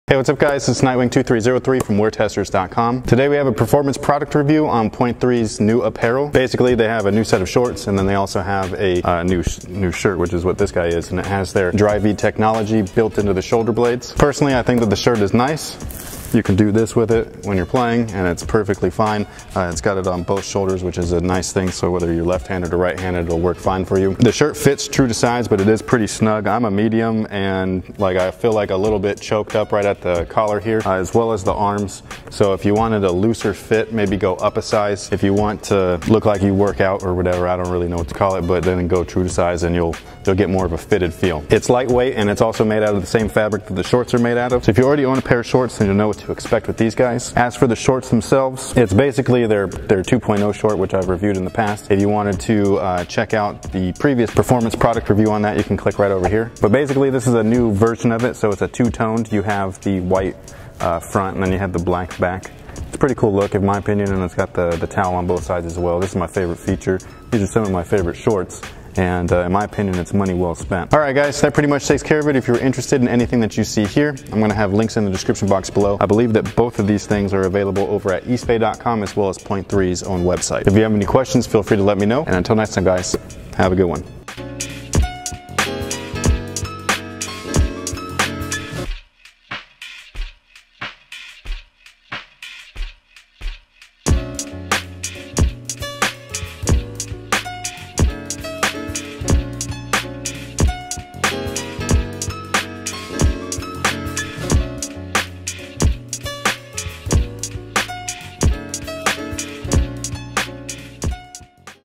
POINT 3 Basketball SNYPER 2.0 | Performance Product Review
Hey Guys! Here is a performance product review on the new POINT 3 Basketball SNYPER 2.0 short sleeve shooting shirt.